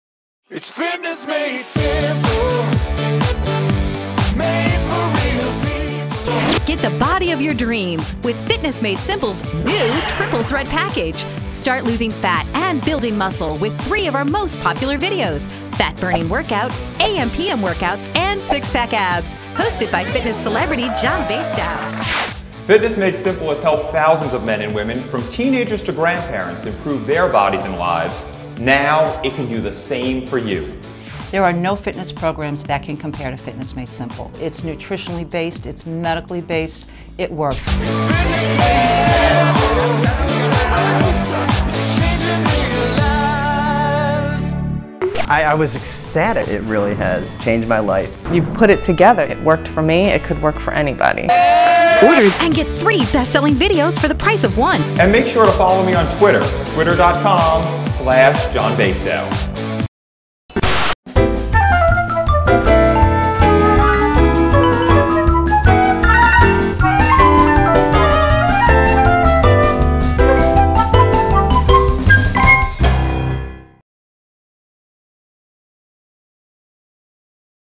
SIX PACK ABS: Lose Fat and Build Muscle
This is a Fitness Made Simple national spot for the 'Triple Threat Package' (3 DVDs for the price of 1), featuring TV fitness personality John Basedow (@JohnBasedow).

for more info on my book and DVDs!